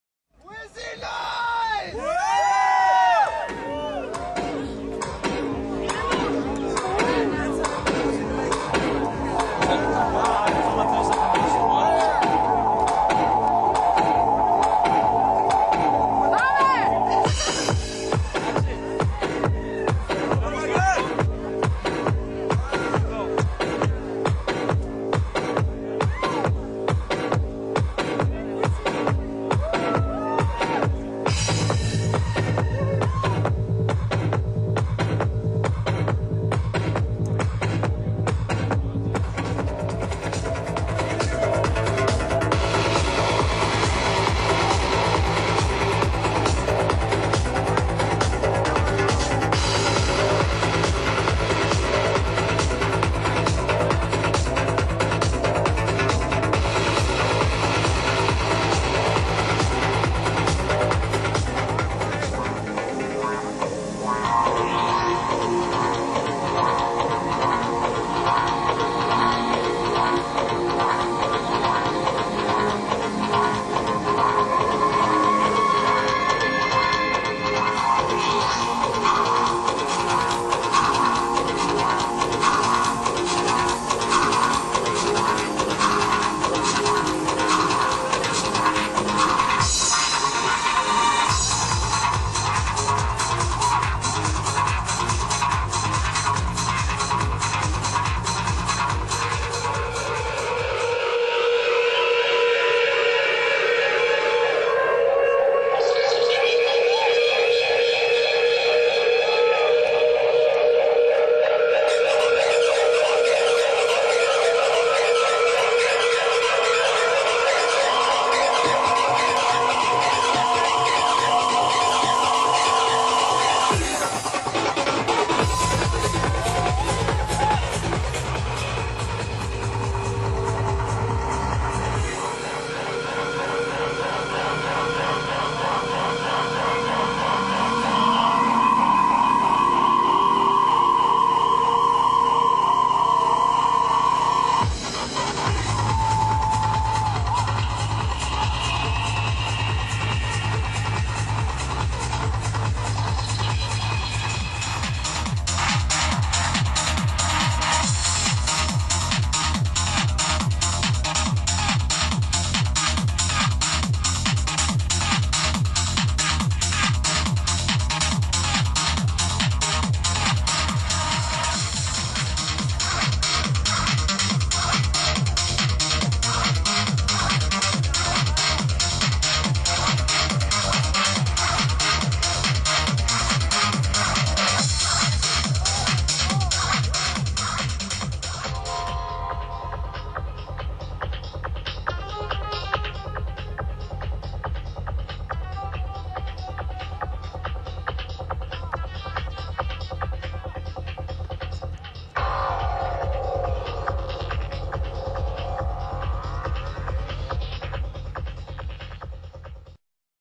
Wizzy Noise & VJ LoveMushroom - Live in Mojave Desert (2003) Psytribe
2003, Psytribe, Mojave desert, California.